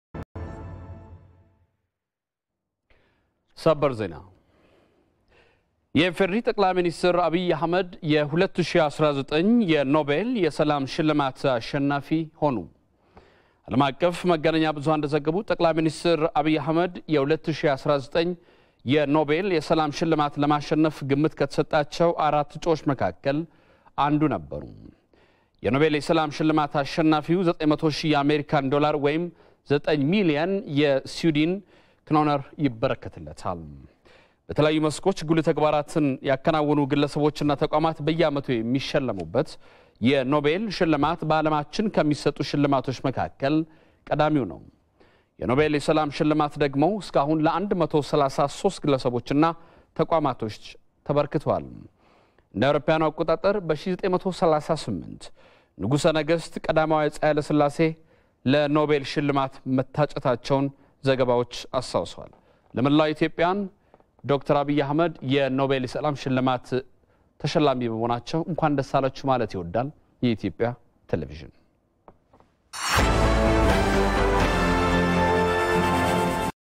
ሰበር ዜና ጠቅላይ ሚኒስትር ዐቢይ አህመድ የ2019 የሰላም ኖቤል ሽልማትን አሸነፉ | EBC
ሰበር ዜና
etv ጠቅላይ ሚኒስትር ዐቢይ አህመድ የ2019 የሰላም ኖቤል ሽልማትን አሸነፉ